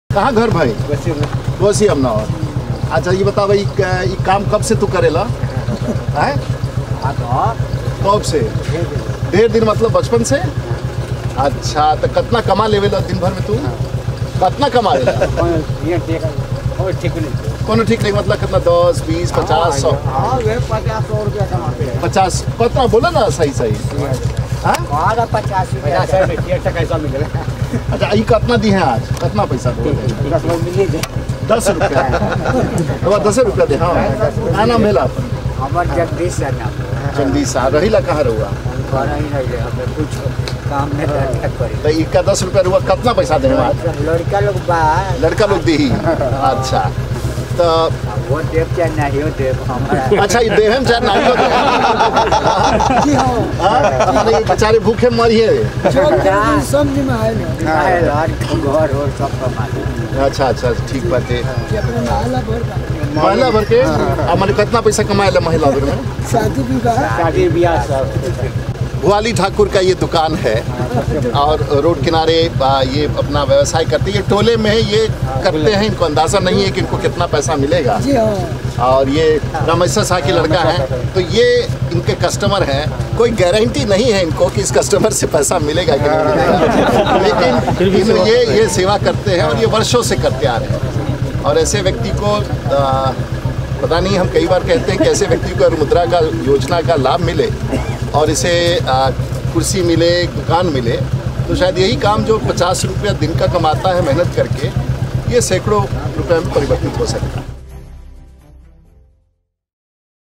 गोसी अमनौर, सारण के भुआली ठाकुर जी से वार्ता...
इनके जैसे कामगार ग्रामीण अर्थव्यवस्था की रीढ़ है जो PMMY के असल हक़दार है। भाईचारा और सौहार्द के साथ व्यवसाय करने वाले ऐसे ग्रामीण ऋण पाकर रोजगार सृजन के साथ ही अर्थव्यवस्था को मजबूती प्रदान करेंगे